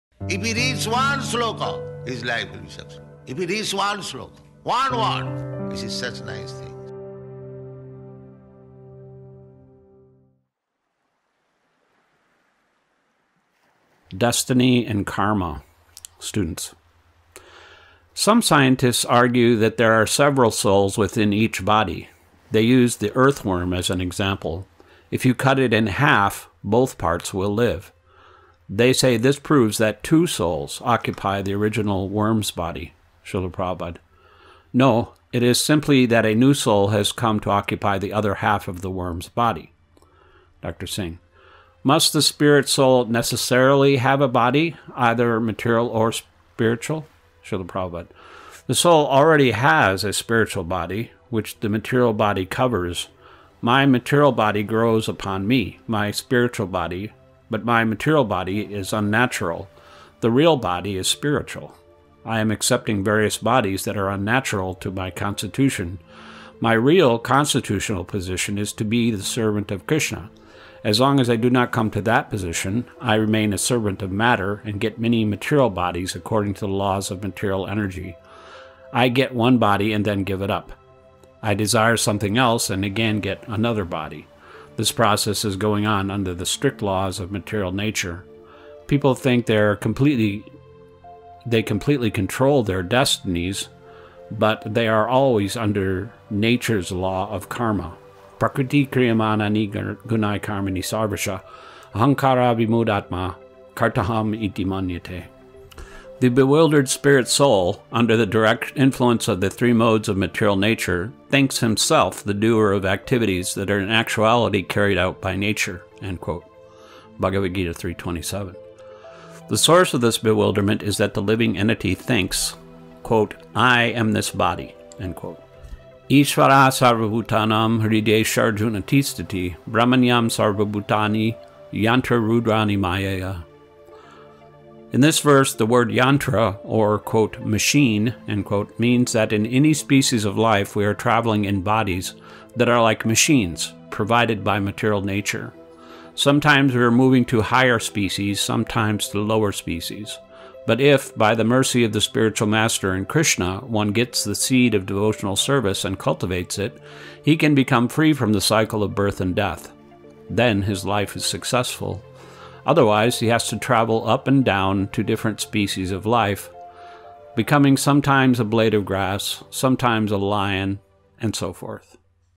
Destiny and Karma / Life Comes from Life
For people who have come to accept every pronouncement of modern scientists as tested and proven truth, this book will be an eye-opener. Life Comes From Life is an impromptu but brilliant critique of some of the dominant policies, theories and presuppositions of modern science.

This book is based on taped morning-walk conversations that Srila Prabhupada had with some of his disciples during 1973, in the Los Angeles area. On those mornings when he focused on science, Srila Prabhupada spoke mainly with his disciple Thoudam D. Singh, Ph.D.

Each day, wherever in the world he happened to be, Srila Prabhupada would go out for a lengthy stroll in the chill quietude of the early morning, and cloaked in a warm wrap, he would share intimate moments with a small group of students, disciples and special guests. Some mornings found him immersed in contemplation or quiet appreciation of the surroundings, and little dialogue emerged. At other times he spoke at great length, and often with considerable intensity, on various subjects. During these animated discourses he demonstrated that philosophical analysis need not be a dull, abstruse affair, but can be a dynamic cutting edge into every sphere of life. Nothing could escape his keen intellect, deep spiritual insight and uncommon wit. Rejecting superficial and dogmatic thinking, he edified, challenged, cajoled, charmed and enlightened his students, and he carefully guided them to increased insight and understanding.

Srila Prabhupada (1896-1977) is an internationally recognized author, scholar and spiritual preceptor, and he is widely esteemed as India's greatest cultural ambassador to the world. In Life
Comes From Life, Srila Prabhupada takes the role of philosopher-social critic. With philosophical rigor, profound common sense and disarming frankness, he exposes not only modern science's methodological shortcomings and unexamined biases but also the unverified (and unverifiable) speculations that scientists present to the trusting public as known fact. Thus Srila Prabhupada breaks the spell of the materialistic and nihilistic myths which, masquerading as science, have so bewitched modern civilization.

Text is read by Siksastaka das